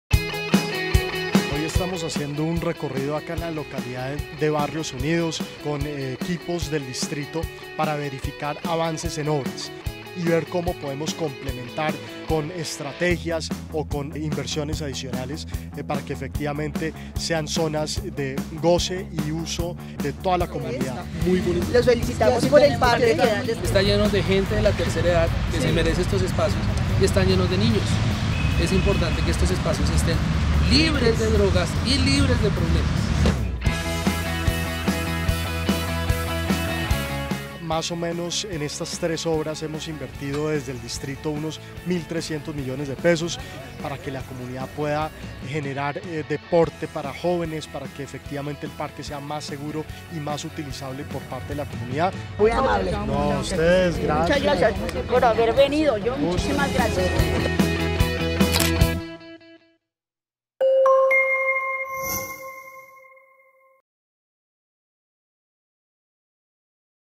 Recorrido localidad de Barrios Unidos - 2019
Recorrimos las obras de infraestructura que están transformando la localidad de Barrios Unidos.

- Invertimos 1.300 millones de pesos